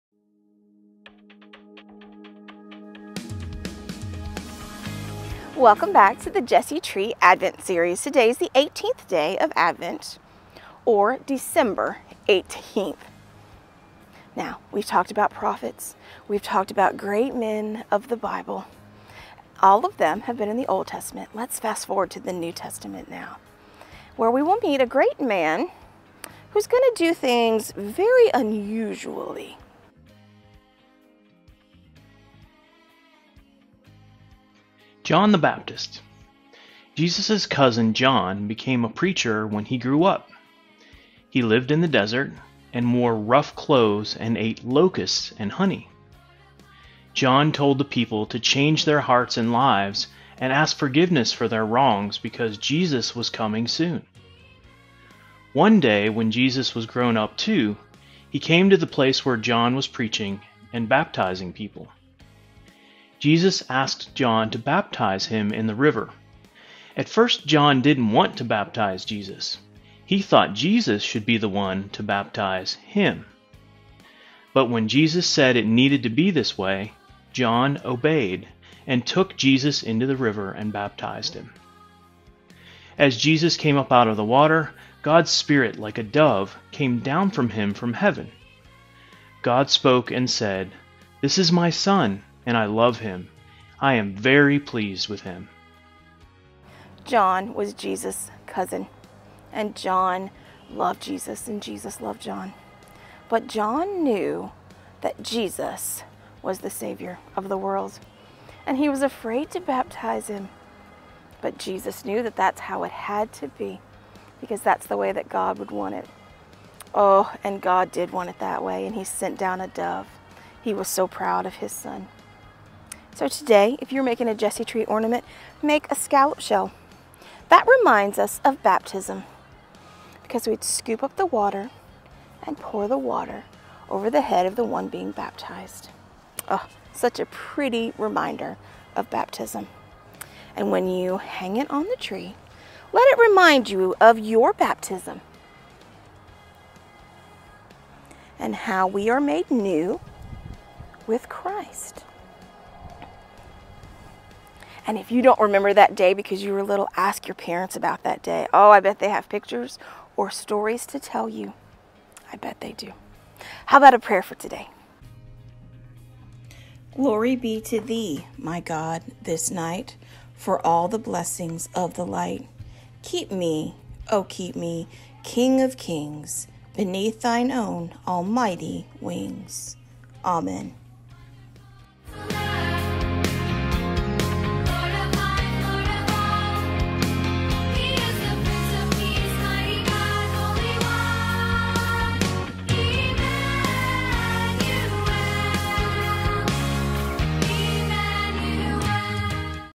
Jesse Tree Advent, Day 18: John The Baptist
Join me this Advent season as we take a few minutes to remember why we celebrate this time of the year. We will mark the 25 days of December from Creation to the Birth of Jesus Christ our Lord. Your child will receive a small devotion, the Bible story, a simple How-To to make the Jesse Tree ornament if desired and a wrap up prayer.  The perfect thing to begin or end your day as a family.